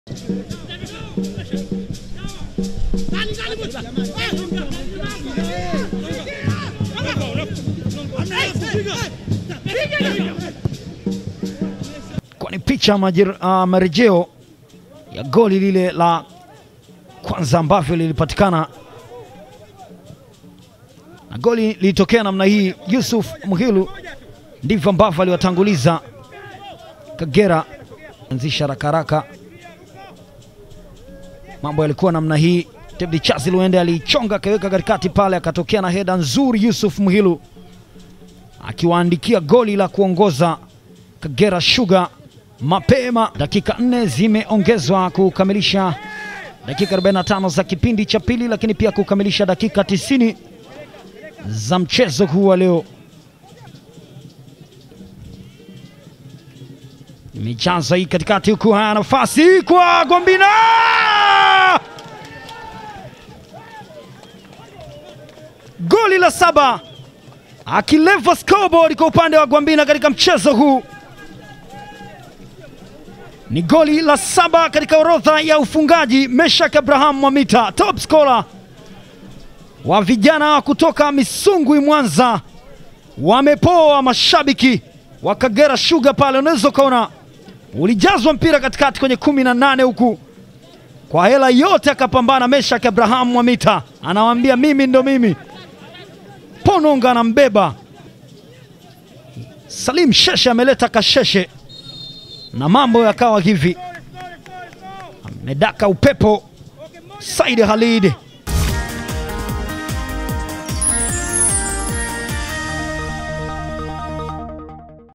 Magoli: Kagera Sugar 1-1 Gwambina FC | VPL 11/02/2021
Goli la dakika ya 90 kutoka kwa Meshack Abraham limeipa Gwambina FC sare ya bao 1-1 dhidi ya Kagera Sugar, katika mchezo wa ligi kuu uliopigwa leo kwenye Dimba la Kaitaba, Bukoba. Kagera ndiyo waliotangulia kupata bao kupitia kwa Yusuph Mhilu.